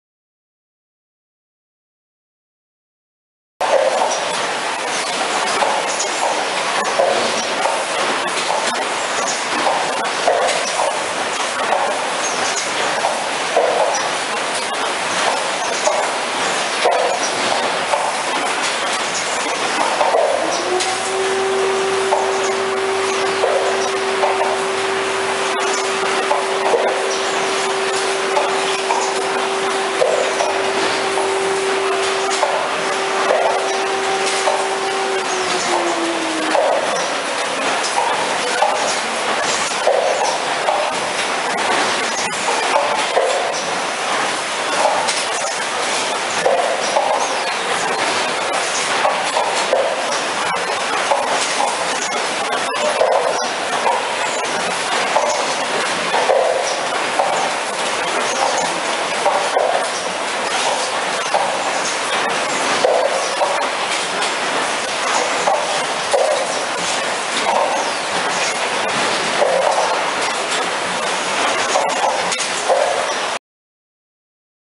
Packaging line for Dairy cups
a complete  automate packaging line for dairy cup.
Capacity - up to 20 Trays/Minute
including:
TR201- Tray Erector.
SHP15 - Top Loader
TS09- Tray Stacker